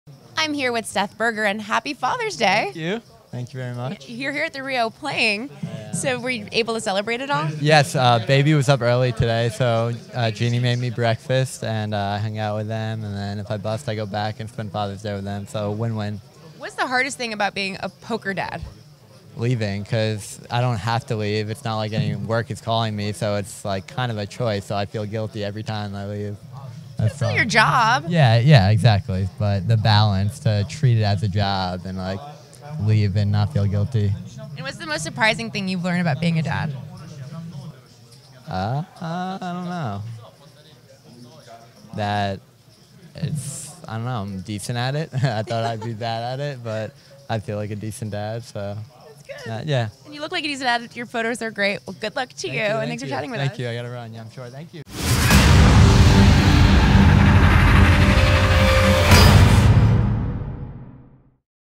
Poker Dad Seth Berger
Seth Berger squeezes in some Father's Day time with his daughter before heading to the Rio